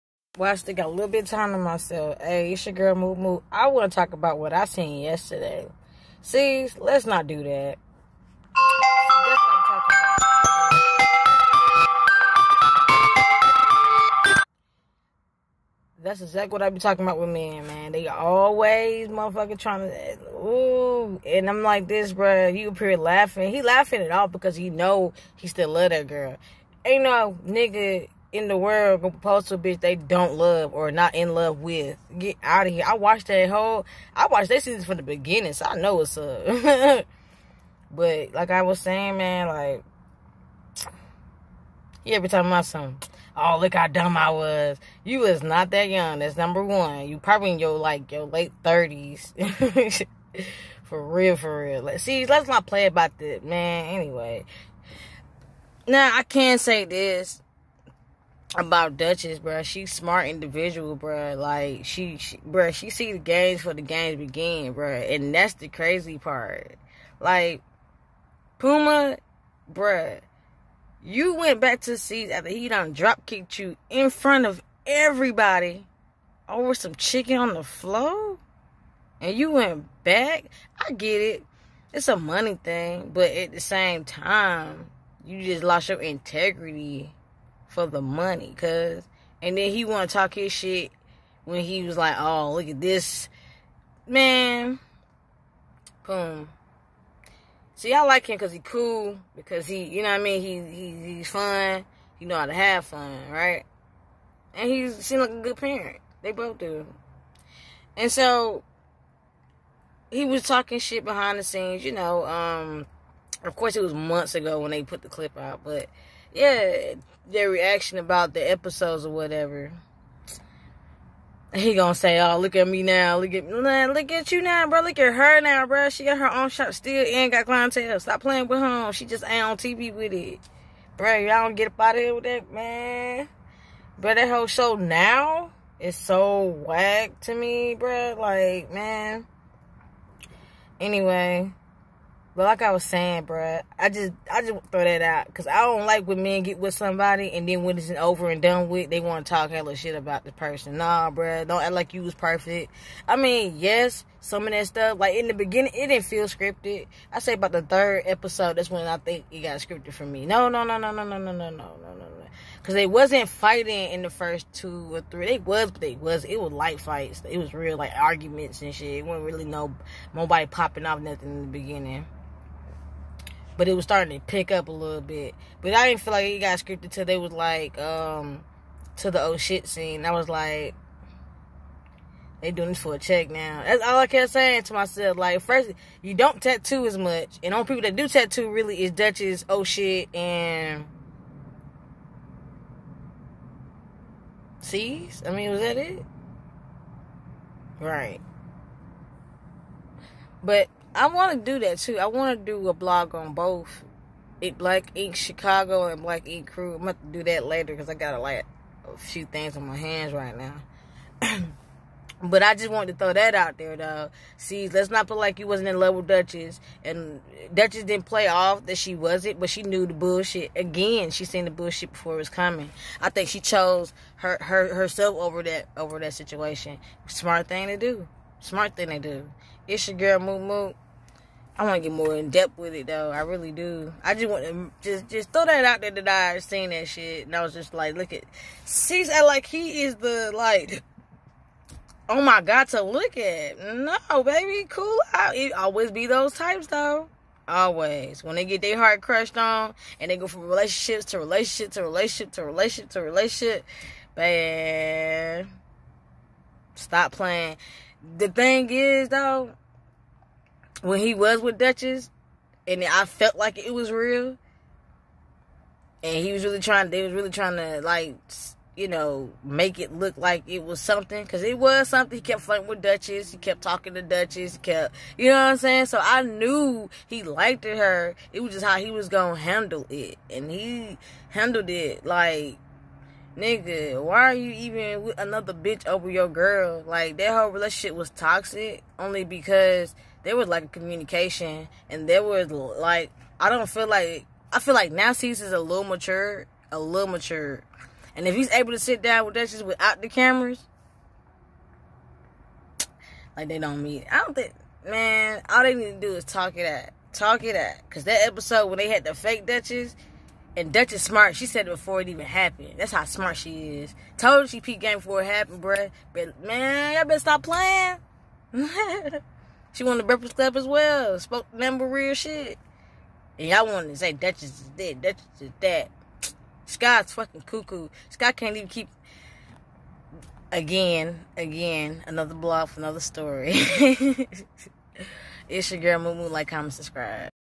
I shouldn't watch clips but I sense Cesar wanna stunt on Dutchess WTF NOT Iz Me Mookie
Cesar be acting funny when his heart broken he looked like he wanted to cry watching it he does love her and wanna be with her the tough act is over bruh 🤣💪🏼💪🏼🎯👀